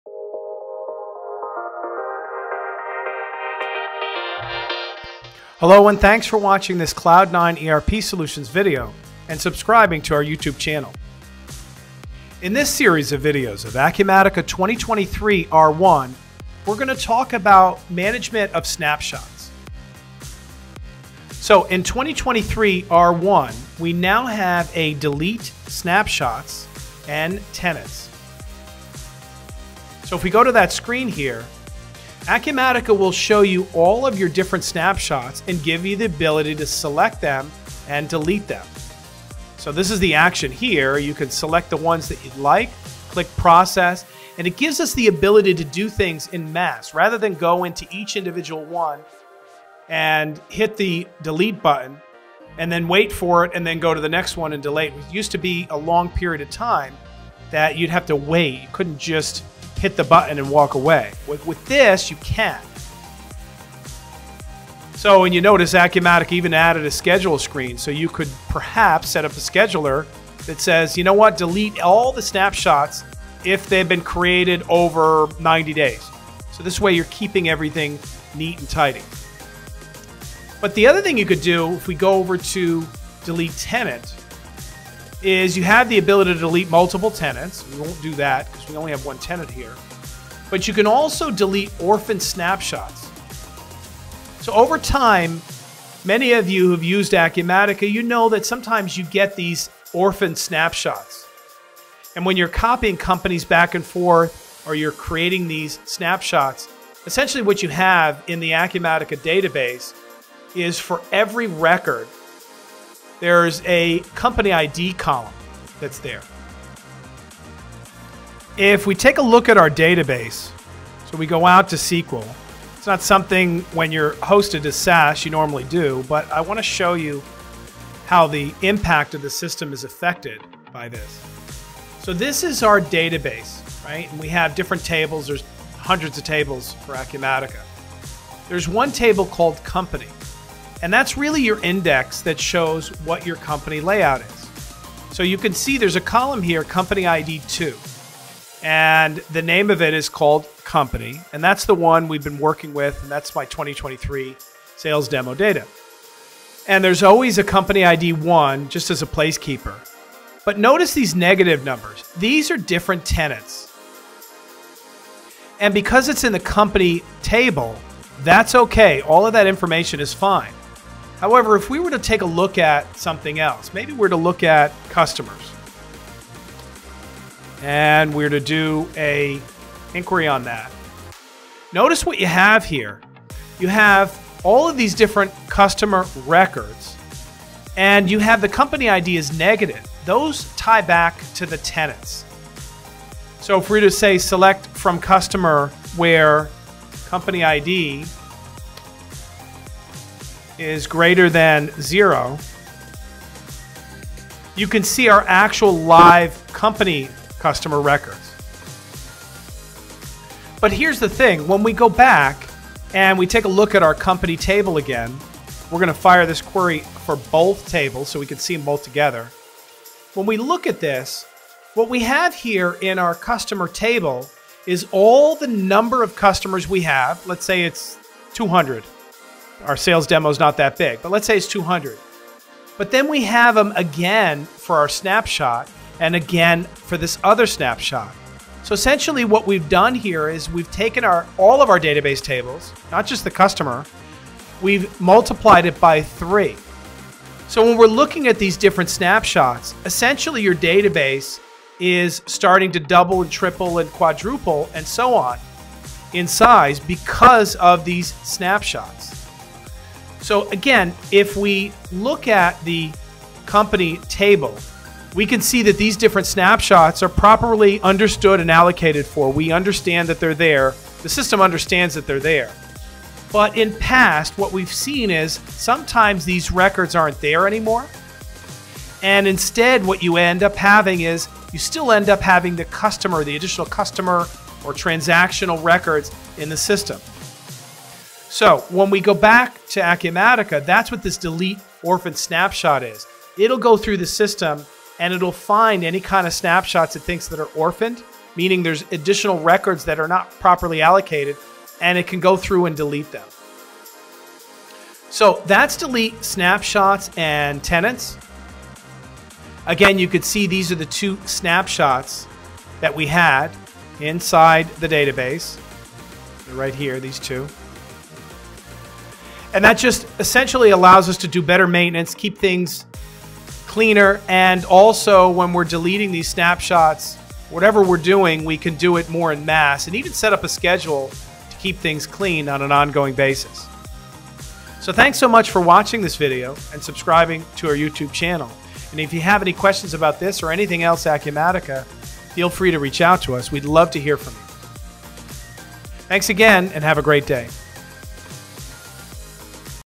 Process Snapshots and Tenants
Maintain your tenants and snapshots!  New 2023 R1 feature makes it easier to keep your Acumatica database cleaner and performing better!